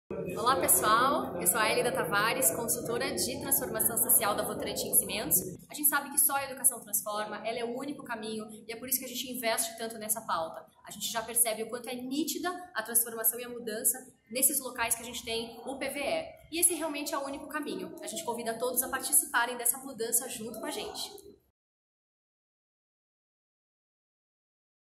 Estar presente faz a diferença - PVE 2019
O programa Parceria pela Valorização da Educação (PVE) existe há 12 anos com um único objetivo: melhorar a qualidade da educação brasileira. E é exatamente esse o convite que a consultora de Transformação Social da @Votorantim Cimentos, Élyda Tavares, faz a todos que acreditam nessa missão.